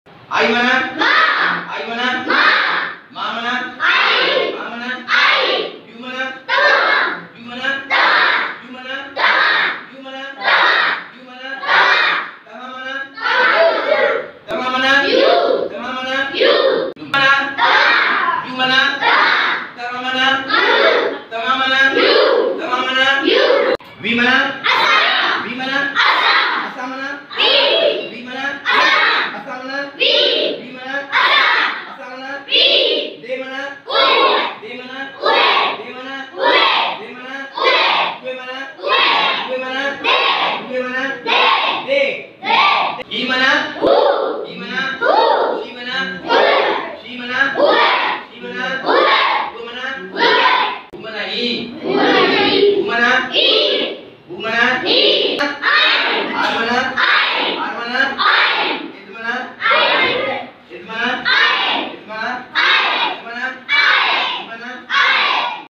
Basic Class activity
اچو ته ٻارن سان ڪجھ ايڪٽوٽيز ڪيون، ٻين تائين شيئر ڪندا پڻ يوٽيوب تي سپورٽ ڪندا @ Sufi Shar